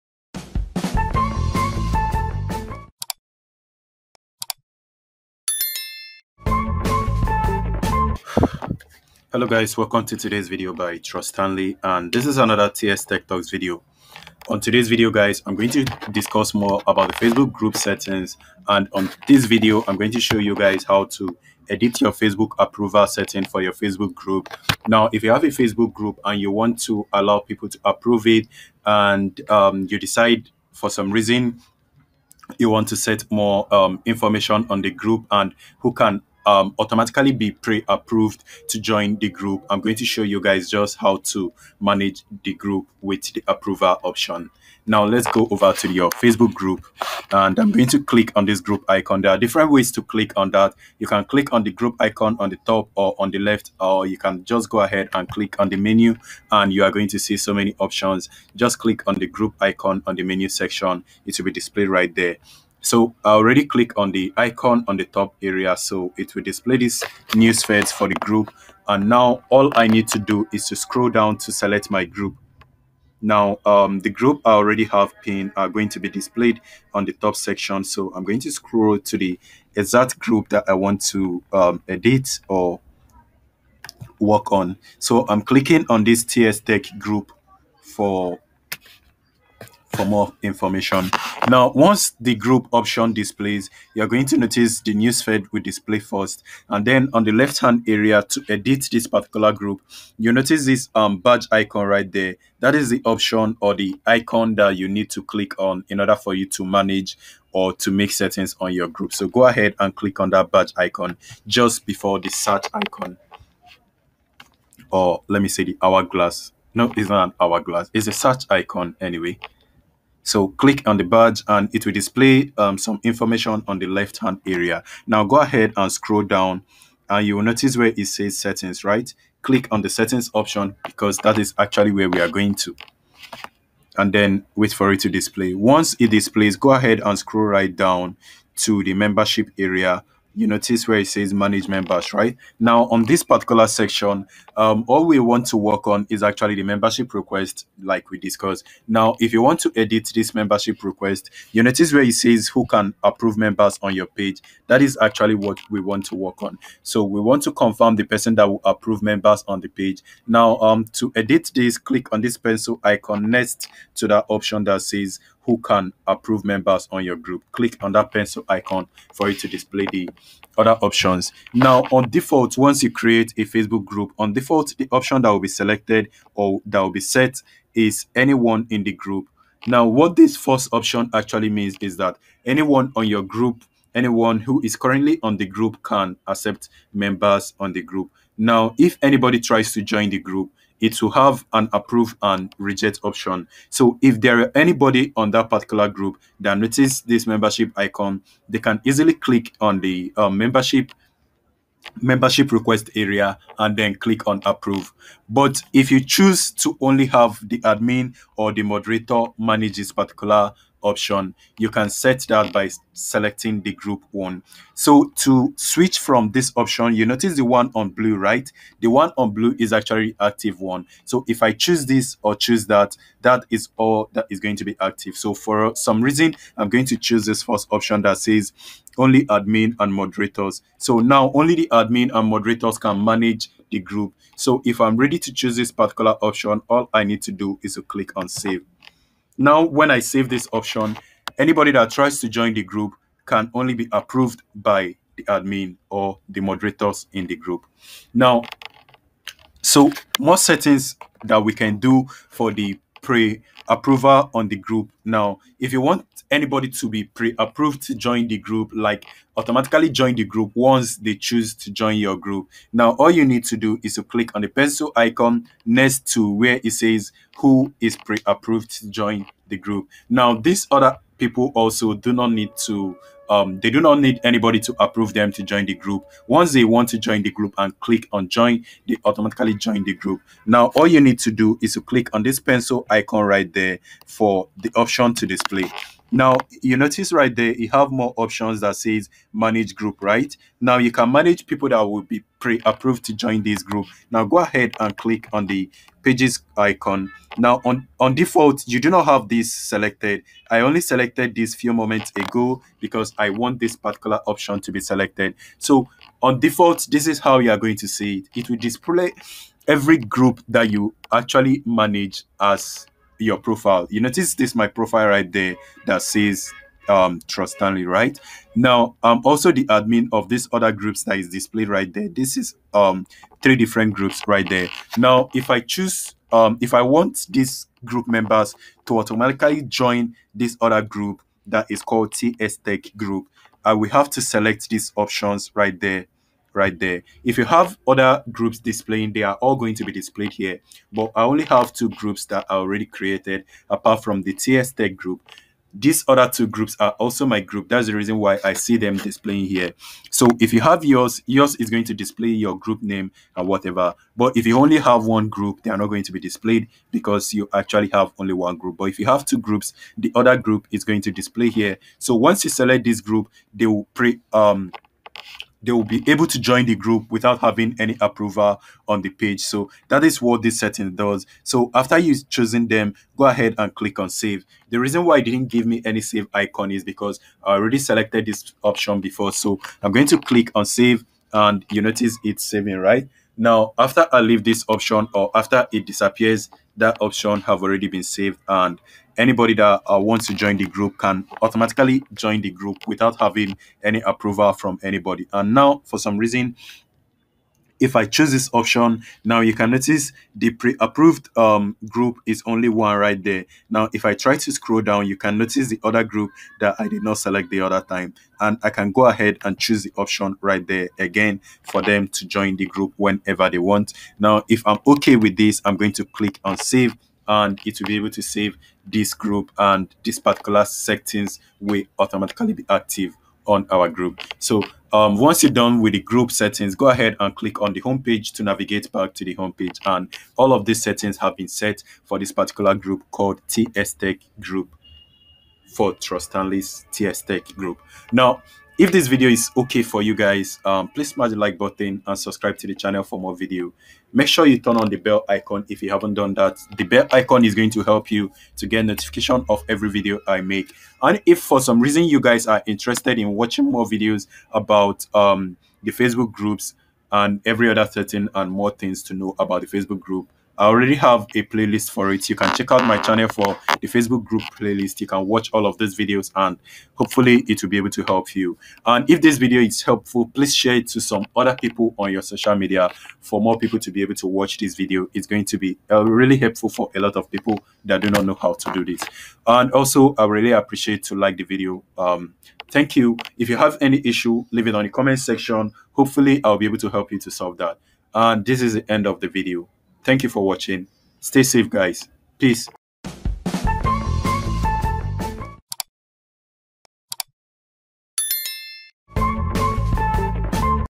how to set only admin approval and more on facebook group
In this video i will show you how to set only admin approval and more on facebook group, you can enable pre-approval of members and you can also set any group member to be able to accept new members into the group at will.